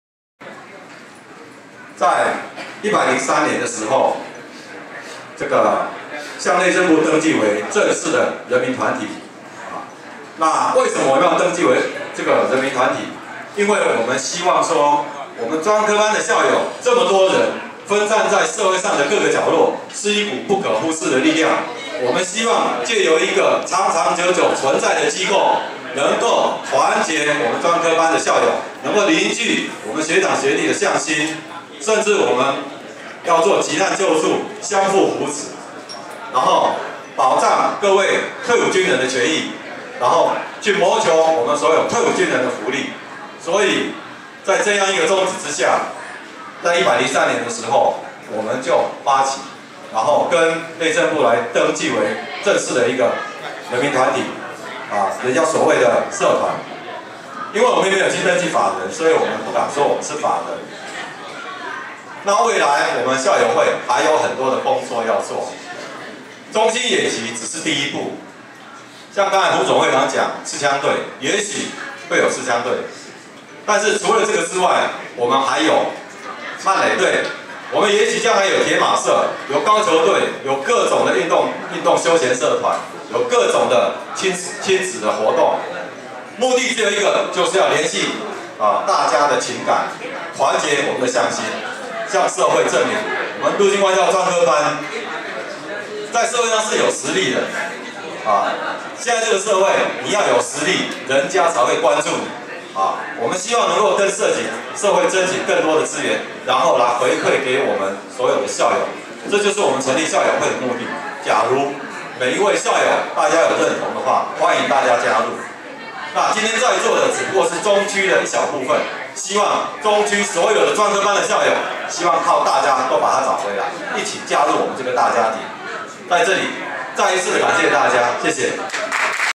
陸軍官校專科班校友會中區分會中興連餐會，詹彥峰致詞
陸軍官校專科班校友會中區分會中興連餐會，秘書長詹彥峰致詞